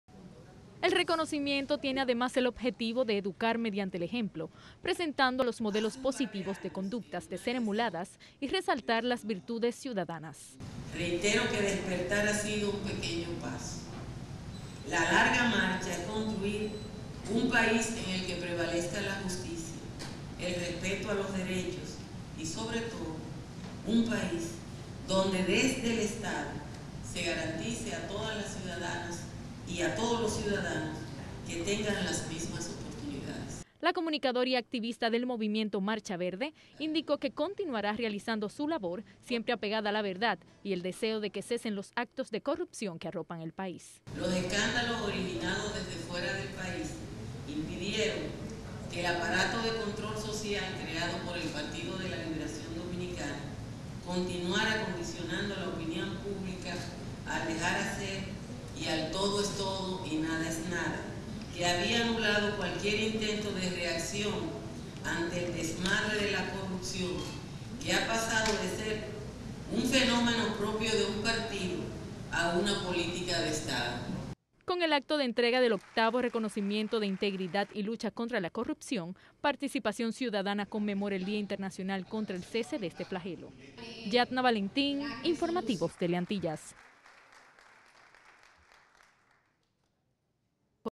Participación Ciudadana reconoce a la periodista Altagracia Salazar
Participación Ciudadana entregó el reconocimiento a la integridad y lucha contra la corrupción 2017 a la periodista Altagracia Salazar, por su trayectoria impecable en los medios de comunicación.